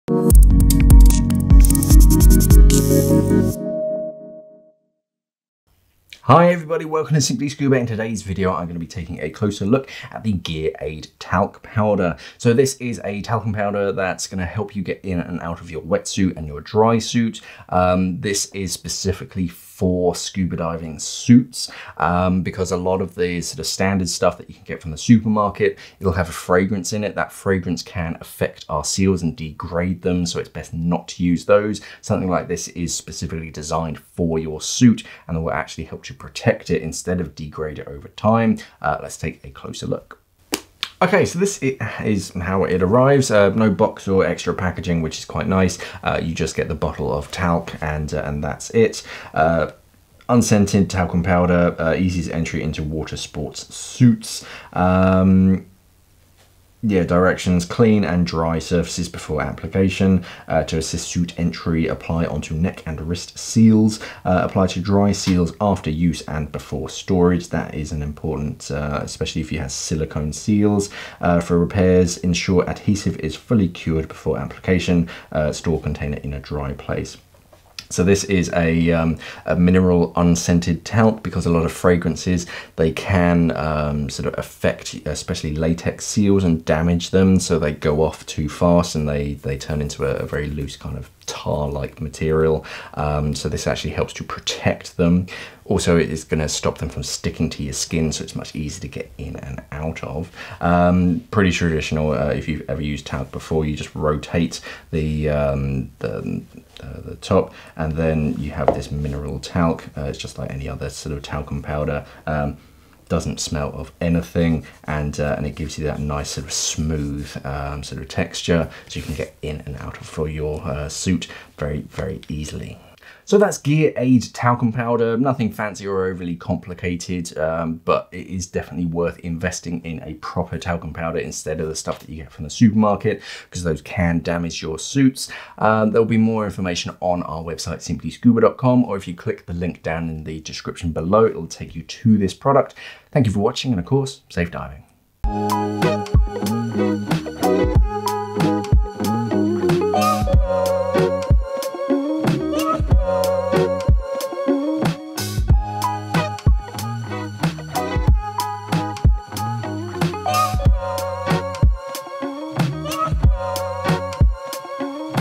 GearAid Talcum Powder | Unboxing
GearAid Talcum Powder is a light, unperfumed talc made from fine French chalk made to make donning your drysuit smoother. Drysuit seals are made to stick close to your skin so when you don your suit before a dive can be tricky and you don't want to pull on the seals too much. A fine layer of Talc will let the seals glide over your hands and seal around your wrist.

Who's Talc For?
Primarily for drysuit divers, talc creates a smooth, dry layer so your drysuit seals glide on easily. The ultra-fine powder disappears over time so it doesn't clog up valves and because it is unscented it won't harm your seals. Other talcs with perfume or scents can affect your seals and break them down so you need to replace them.

Timestamp
00:00 - Hello
00:40 - Unboxing...I guess
01:17 - Closer look
02:02 - K-Bye

Here at Simply Scuba, we're all about giving you the best advice, information and news about the awesome world of scuba diving.